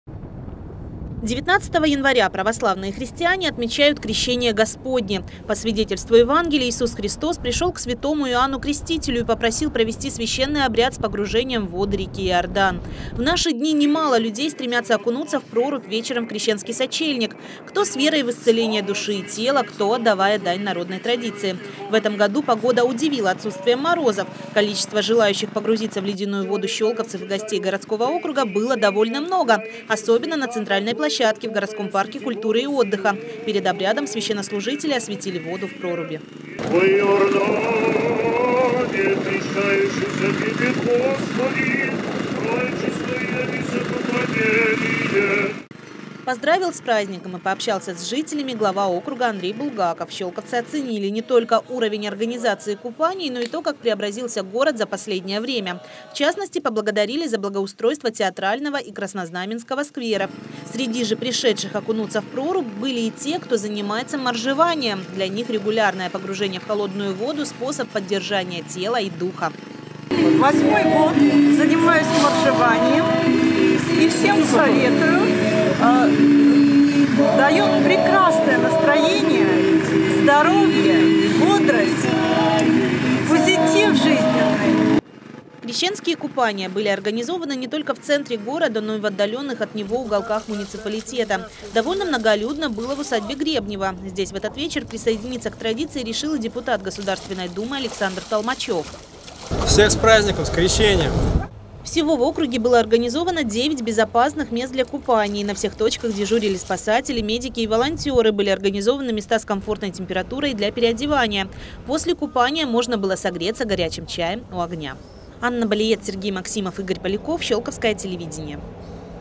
Крещенские купания прошли в городском округе Щёлково
Щёлковцы приняли участие в Крещенских купаниях. Окунуться в ледяную воду можно было как в городе, так и в отдалённых от центра населённых пунктах. Всего было оборудовано девять мест.